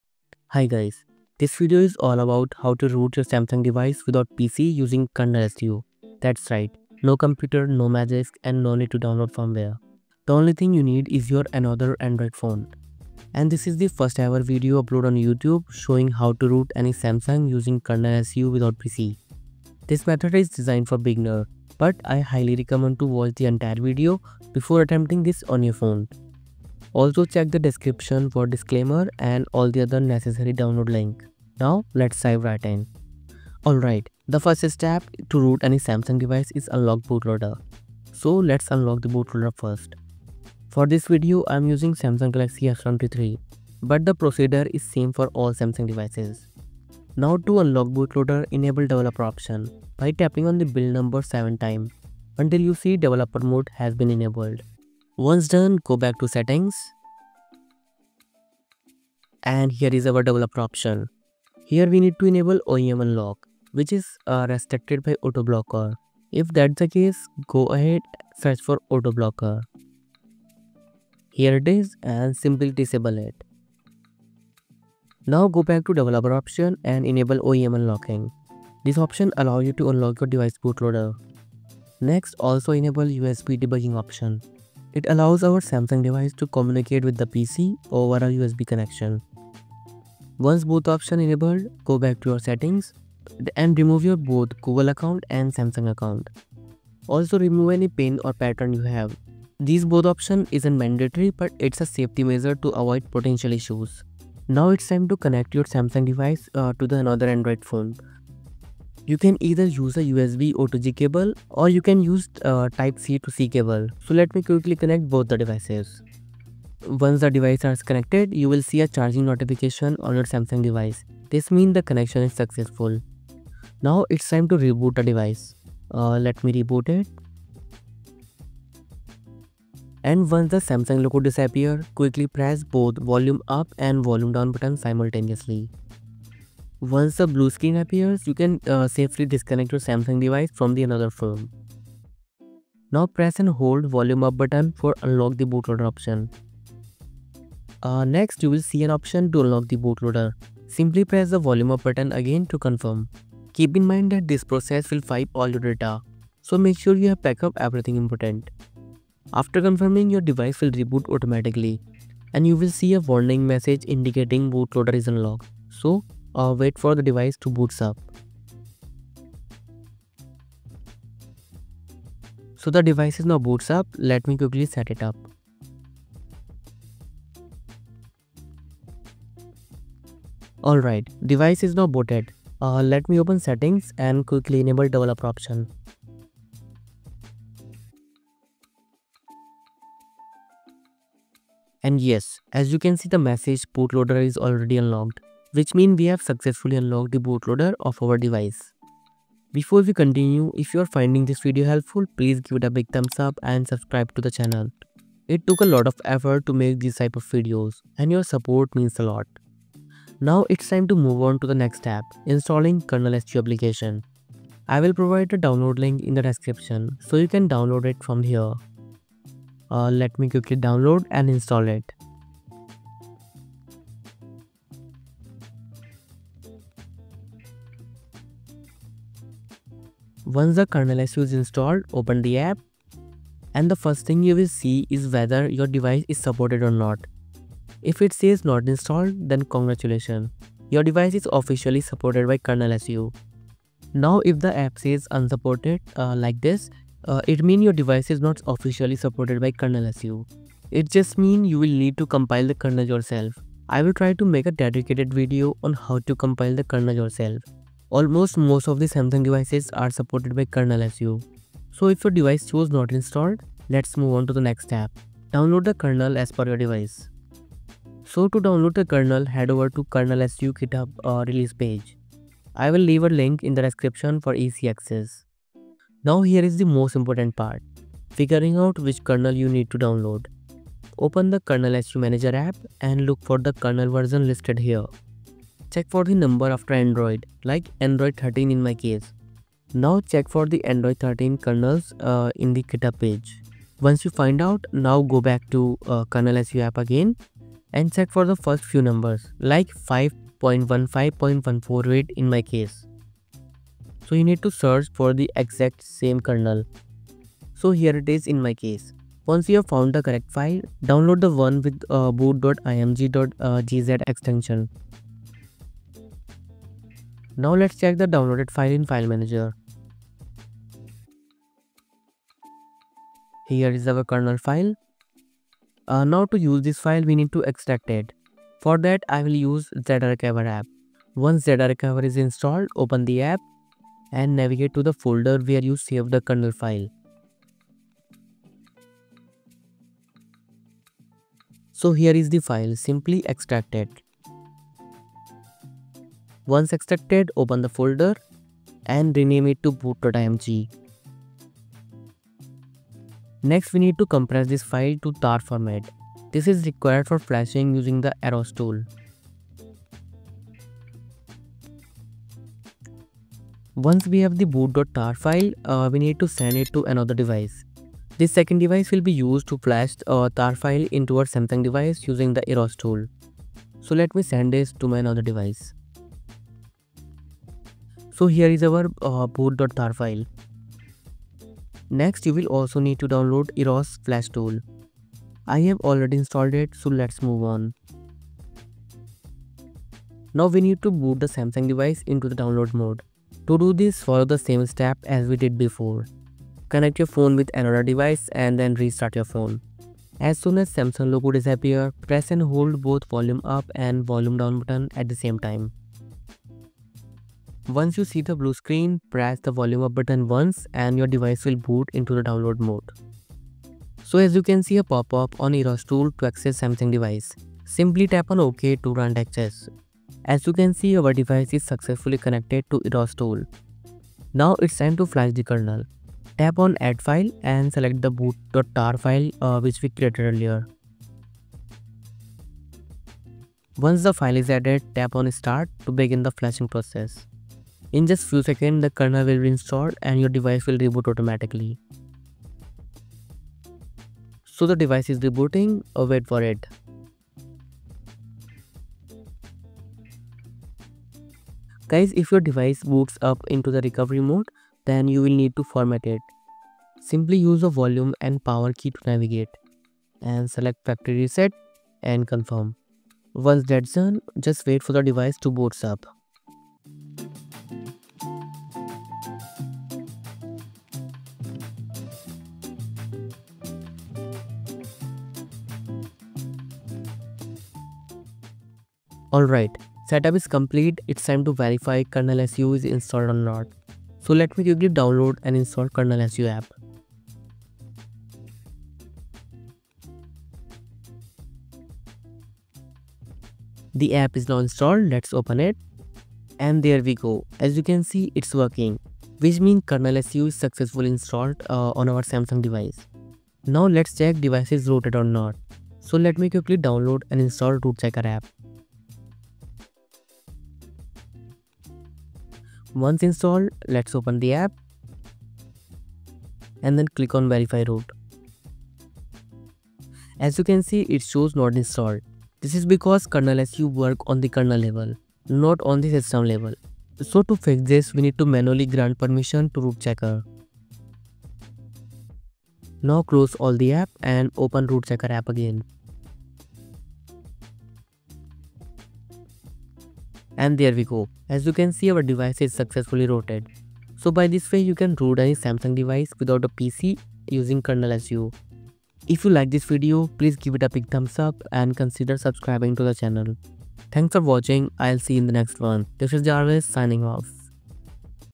How to Root Samsung Without PC with KernelSU | Root without Magisk, without TWRP (2026)
Install KernelSU in Samsung device. Learn how to root any Samsung device without PC using KernelSU or KSU! Without Magisk, without TWRP and no firmware downloads, just another Android phone! Learn how to unlock the bootloader, flash custom kernels, and configure modules like Play Integrity Fix and Zygisk to bypass root detection, all while avoiding FRP locks and data corruption risks.

Special thanks to Gabriel2392 and his ErosFlashTool for making this breakthrough possible.

📌 Timestamps:
00:00 -  Intro  (Root Samsung Using KernelSU)
00:33 -  Unlocking Bootloader
03:30 -  Installing And Download KernelSU
06:30 -  Flashing Kernel with ErosFlashTool
07:58 -  Verifying Root Access